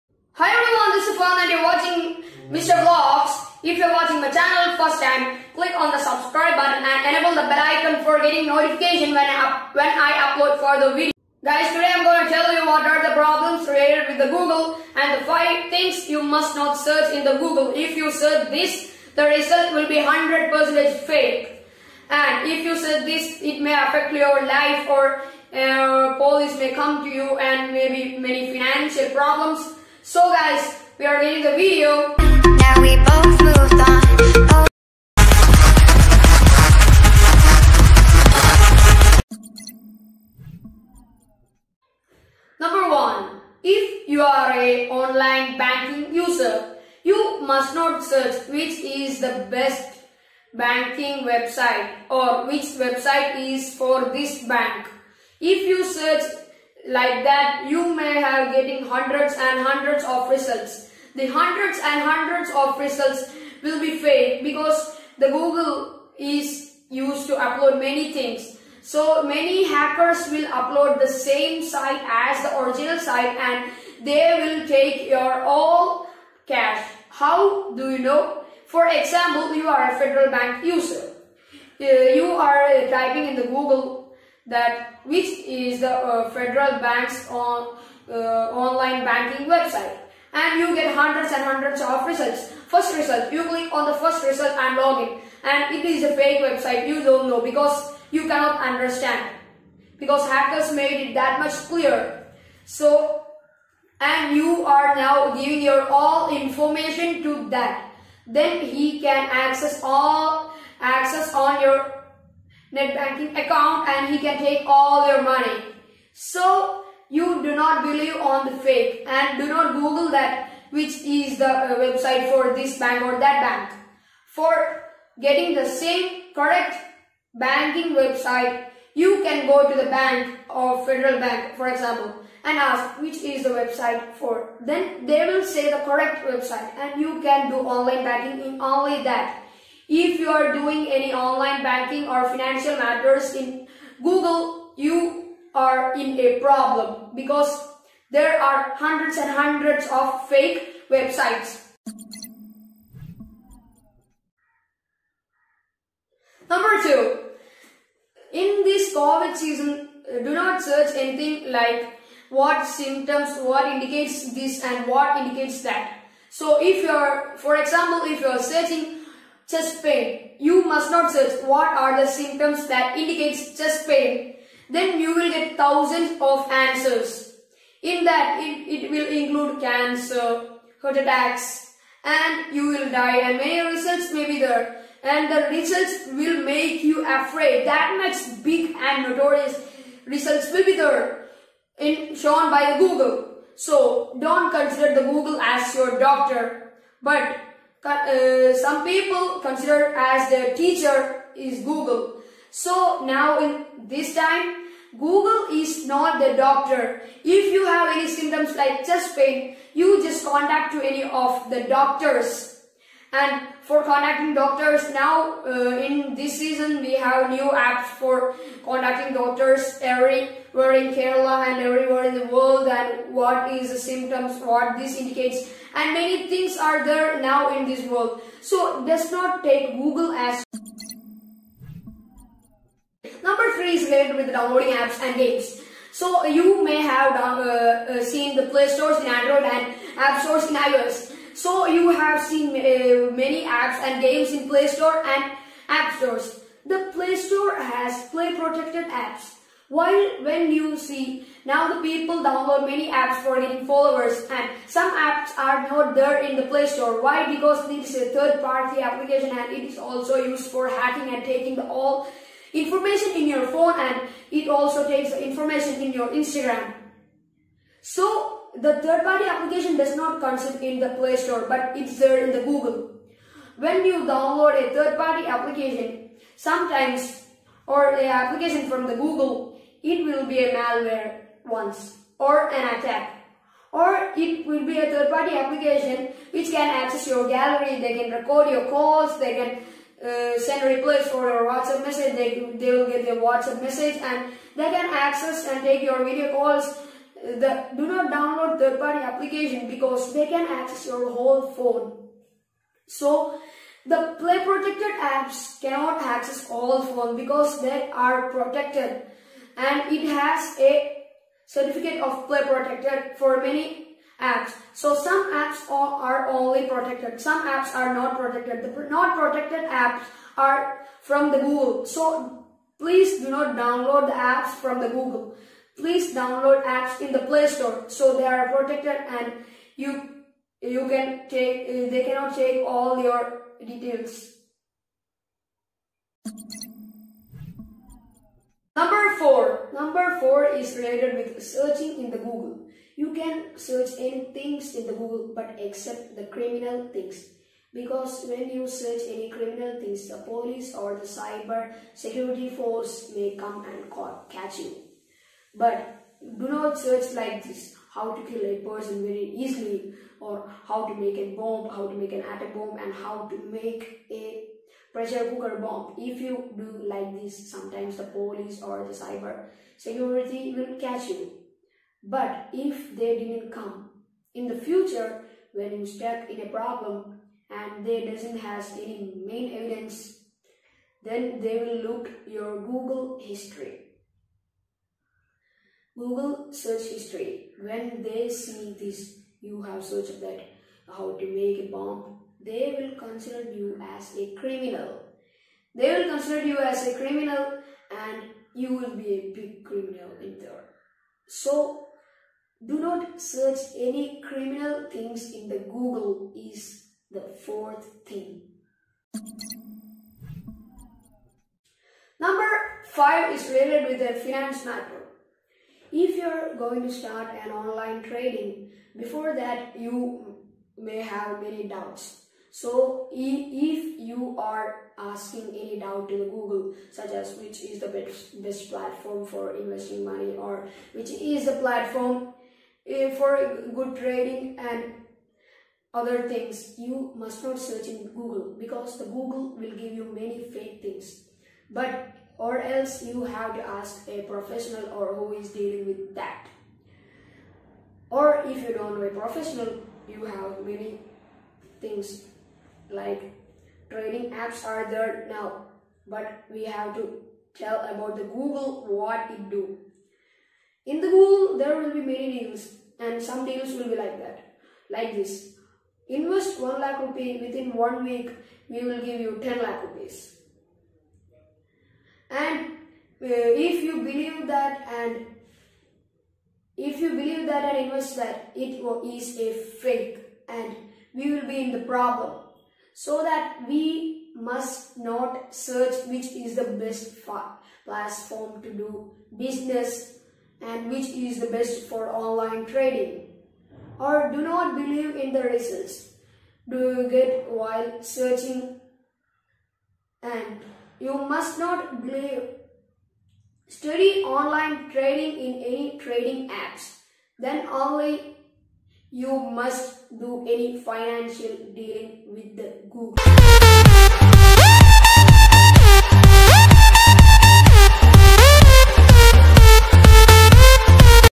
What are the 5 things you must not search in Google 😱 by MR vlogs
In this video I am telling about 5 thing you must not need to search in the Google because if you need the reason pls watch full vedio and subscribe the channel like comment and SHARE